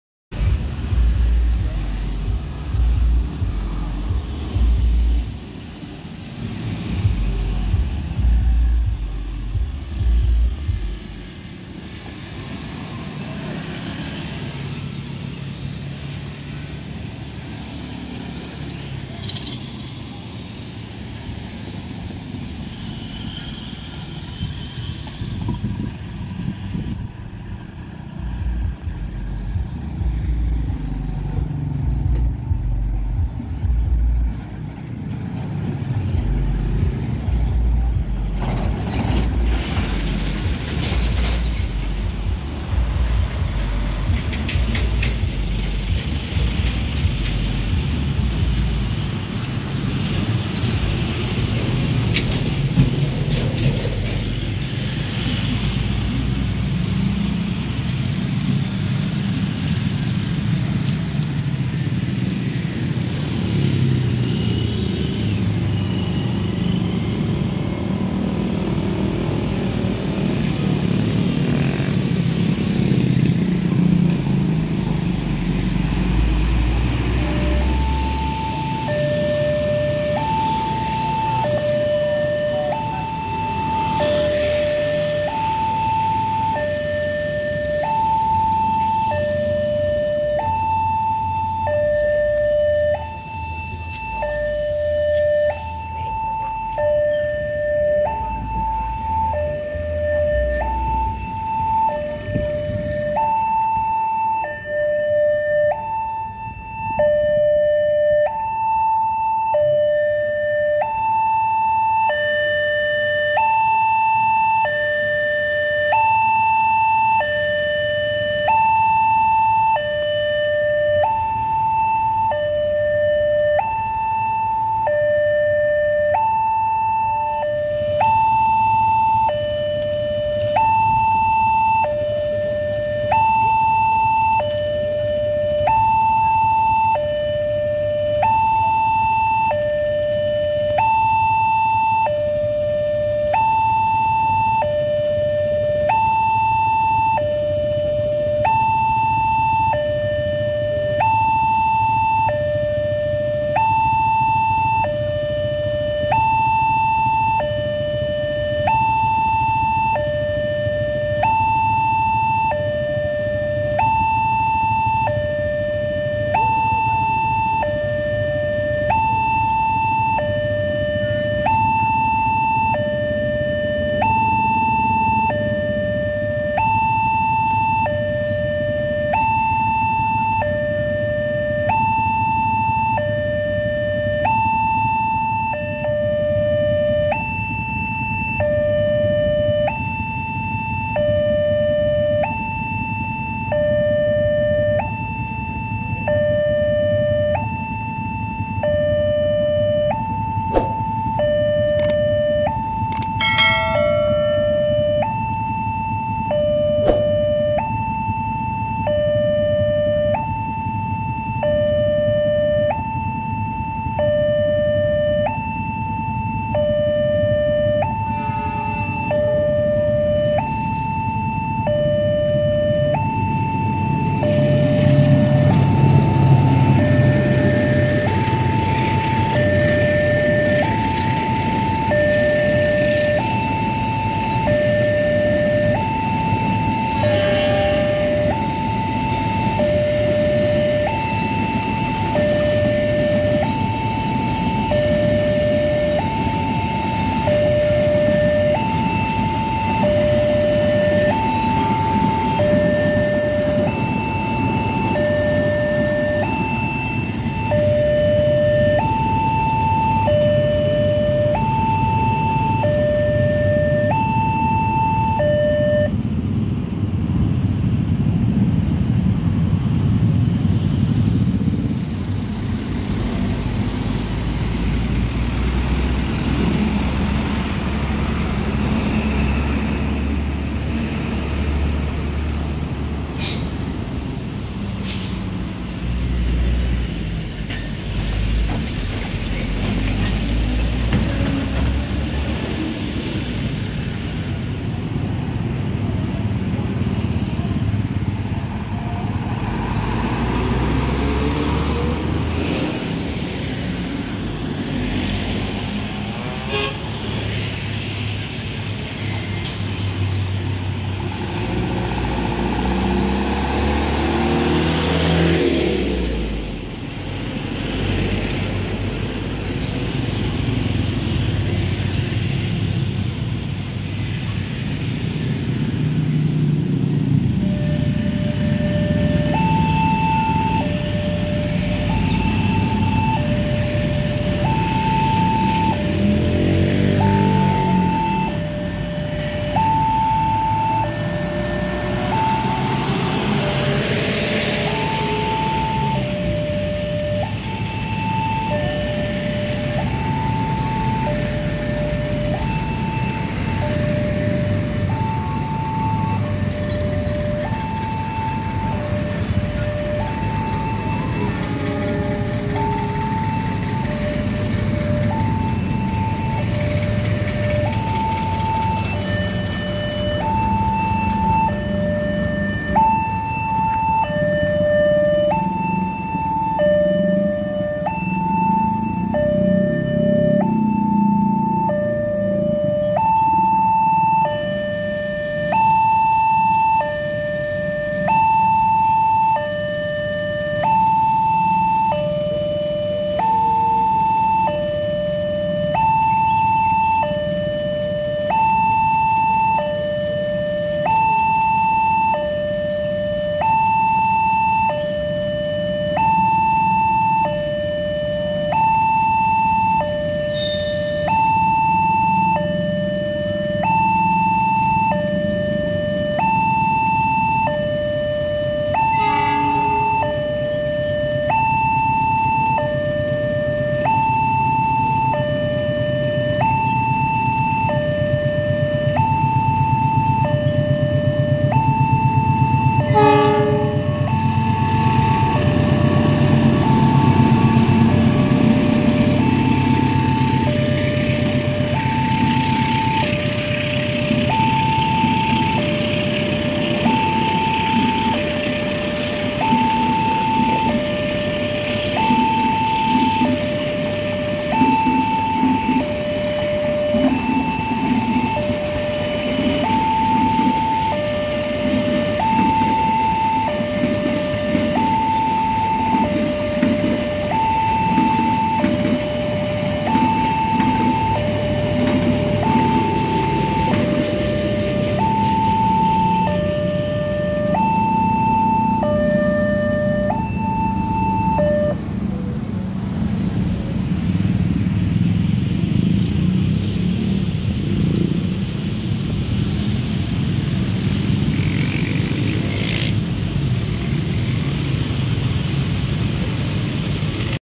Rush Hour | Railroad Crossing Indonesia | Perlintasan Kereta Api Babat, Lamongan
Perlintasan kereta api JPL 253, Jl. Raya Babat - Jombang. Sisi barat Stasiun Babat Kab. Lamongan Jawa Timur Indonesia. JPL ini juga satu-satunya palang kereta yang di tambahi lampu Strobo jadi lebih keren,terang dan unik.